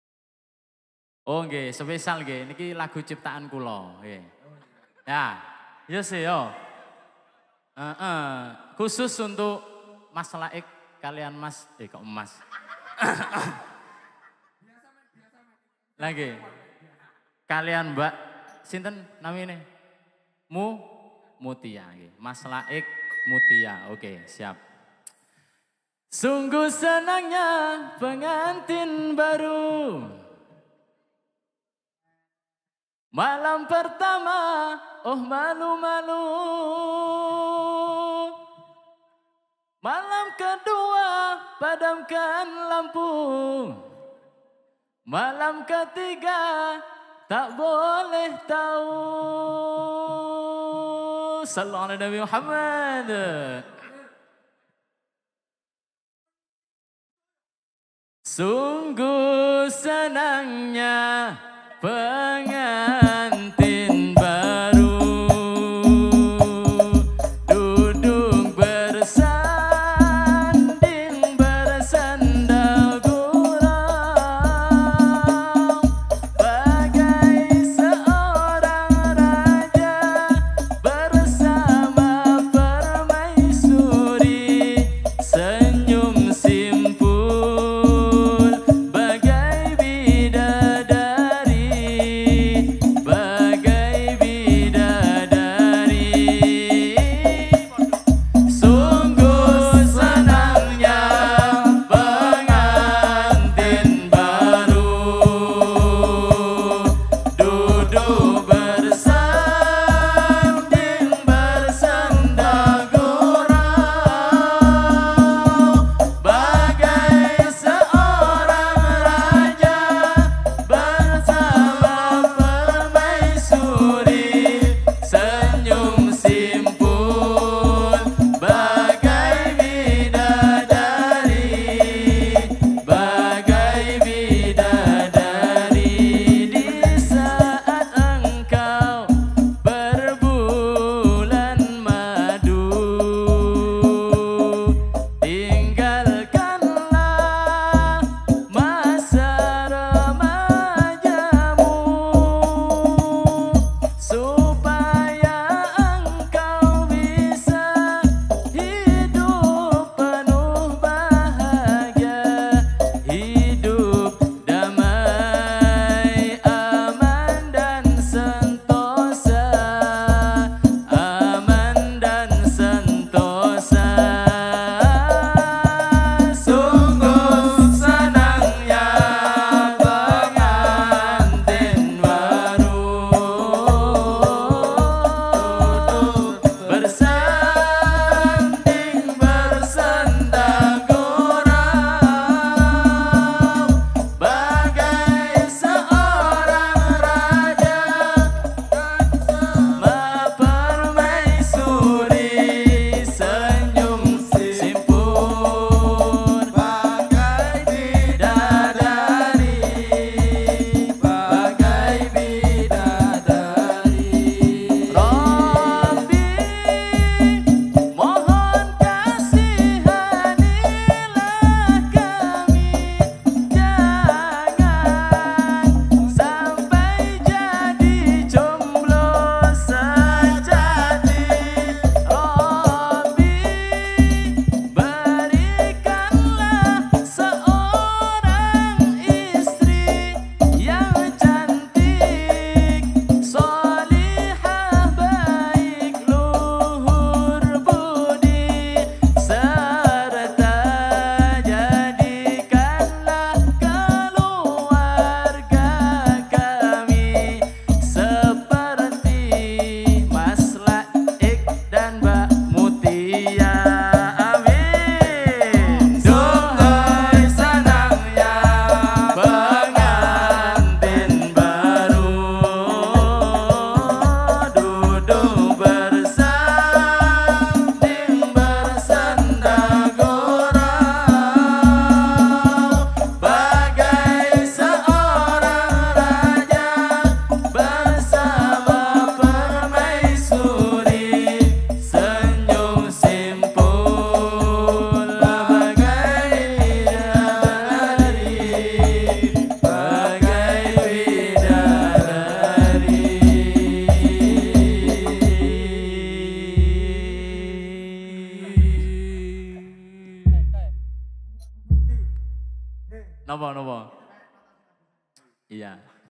CAK FADHLI NIH BOZZZ - Ar Roudhoh || LS Malam Cinta 2021
Dokumentasi Official Lailatus Sholawat Malam Cinta
Menjemput Bahagia Dengan Mulia Dalam Rangka
Pernikahan Gus Laiq & Neng Muthi
Asrama Al-Qurtubi PPBH Mojogeneng – Jatirejo – Mojokerto

Dilarang Keras Upload Ulang Dalam Bentuk Apapun Tanpa Se Izin Dari
"Youtuber Sholawat Indonesia"